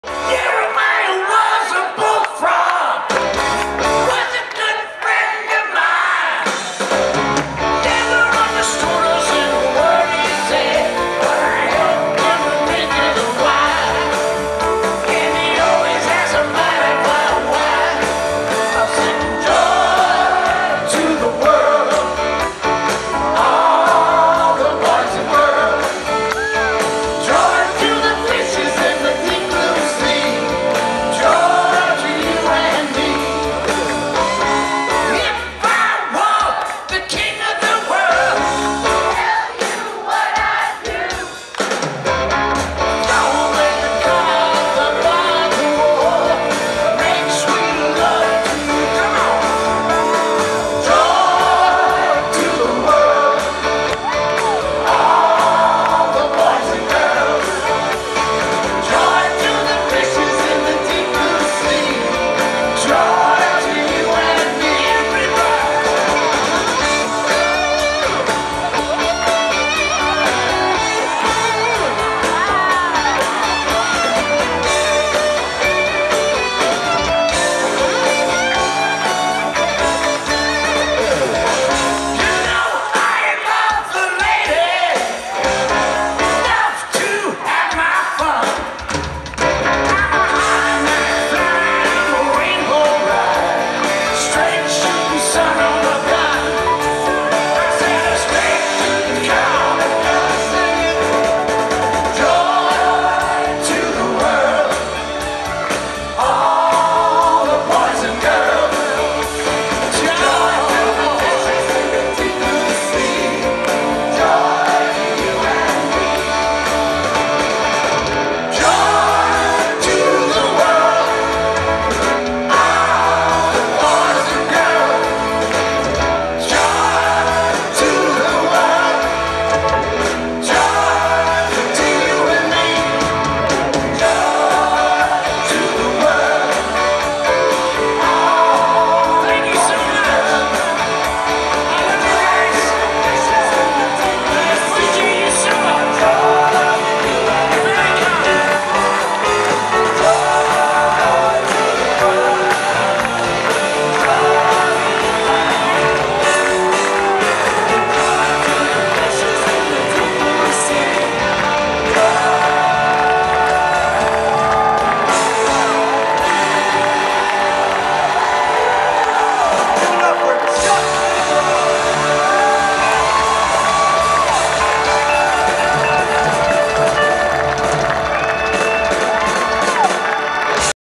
Chuck Negron of Three Dog Night
at the Happy Together Tour 2016 at Thunder Valley, CA
 Video By: Morpheous Music